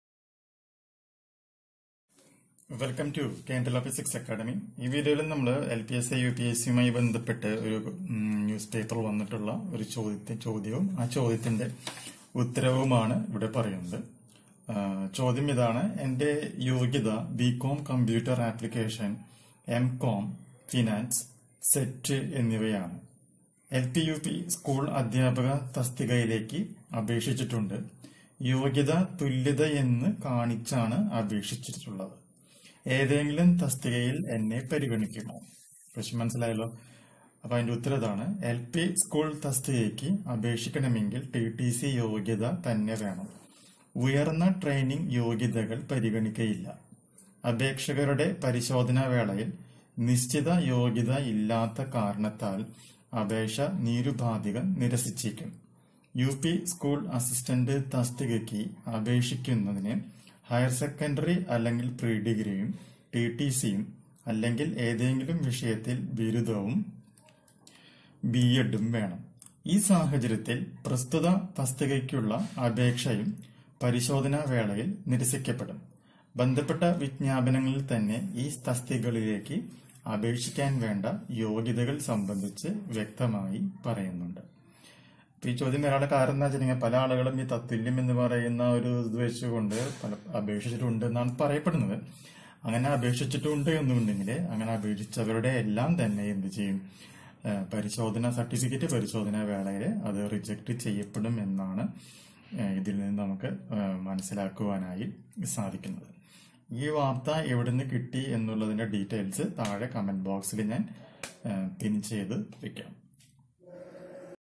LPSA|UPSA Qualification details|ഉയർന്ന യോഗ്യത ഒരു പ്രശ്നം ആകുമോ|lpsa upsa notification 2022
Educational video regarding the qualification of LPSA and UPSA teaching post.In this Q and A video,i clarify a doubt about the qualification@AQLORA1